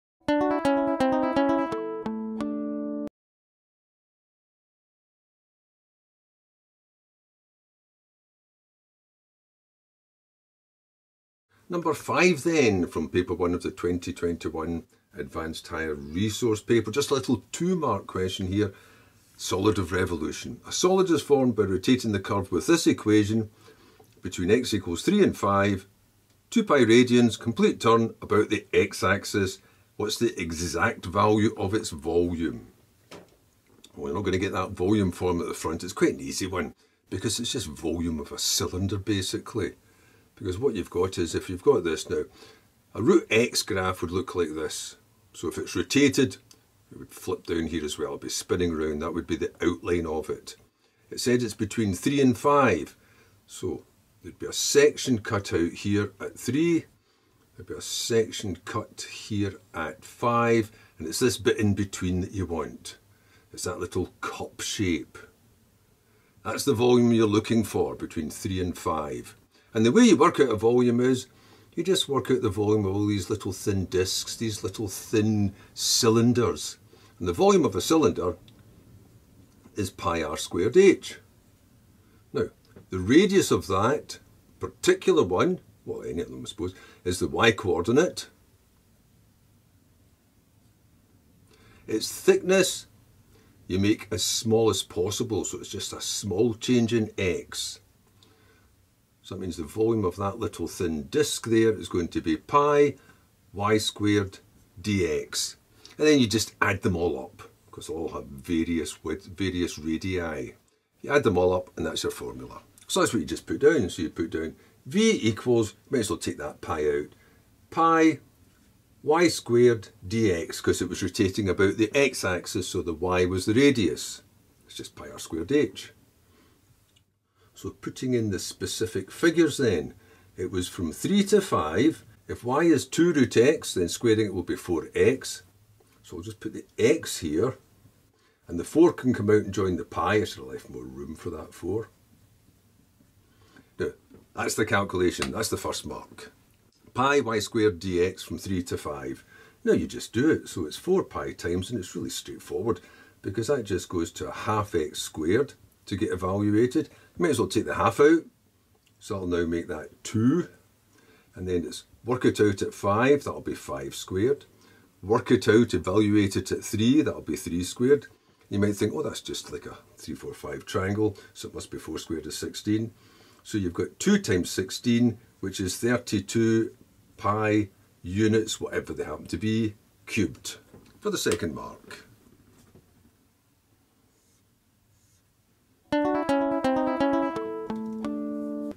21 SQA Adv. Higher Maths Paper 1 No.5 Volume of revolution
Find the volume of a solid formed by rotating a given curve about the x-axis.

Instructional exercise consisting of question 5 from Paper 1 of the 2021 SQA Advanced Higher Mathematics assessment resource.